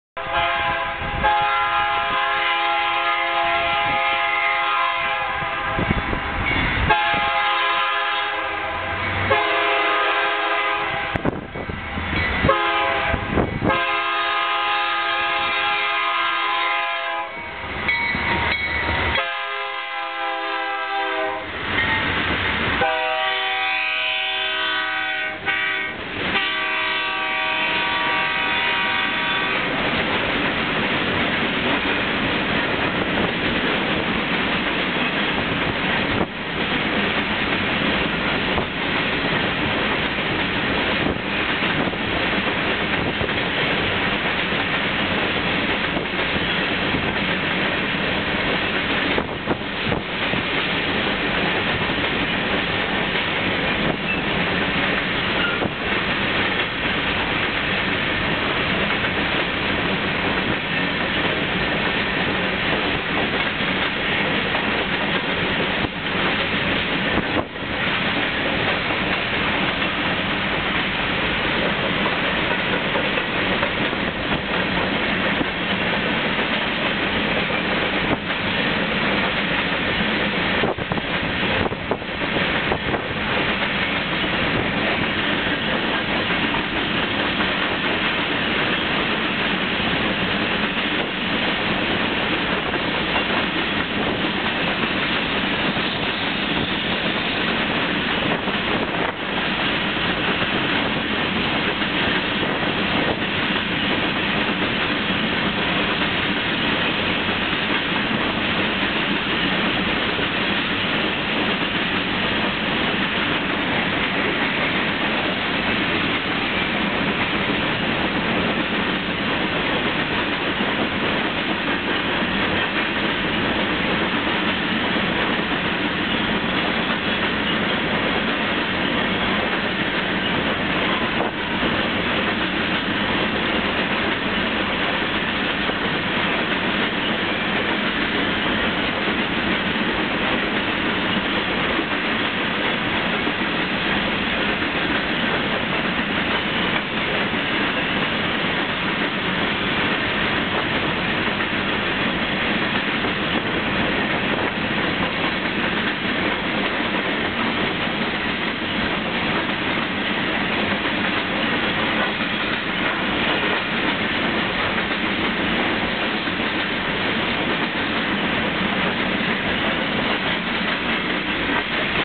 Northbound Grain Train - 08/01/06 - Cleburne, Texas
Power for this train was provided by BNSF 4803 (C44-9W H2) and BNSF 922 (C40-8W H2).